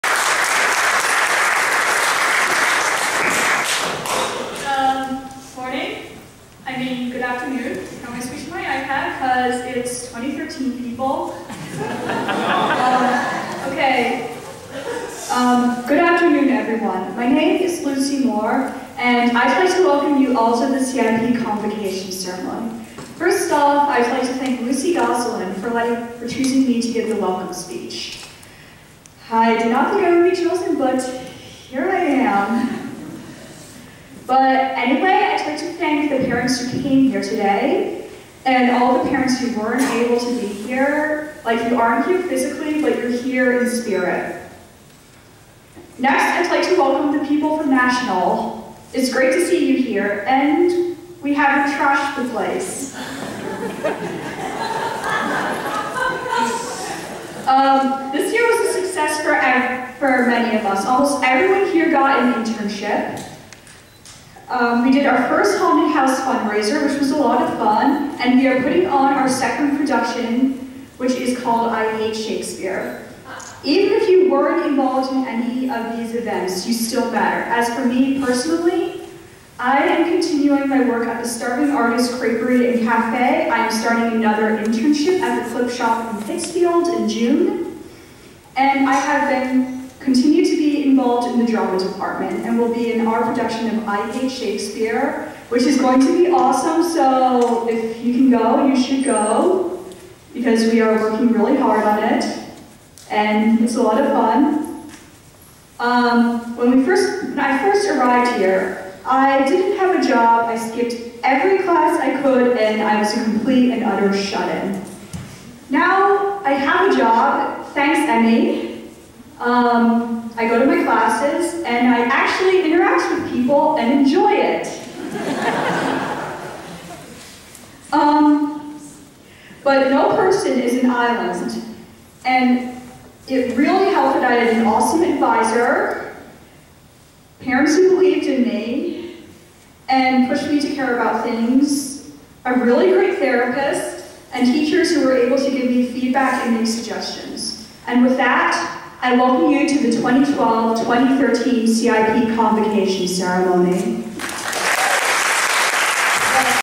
Lucy Convocation Opening 2012 2013
CIP Berkshire student Lucy gives a welcoming at the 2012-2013 Berkshire convocation and award ceremony.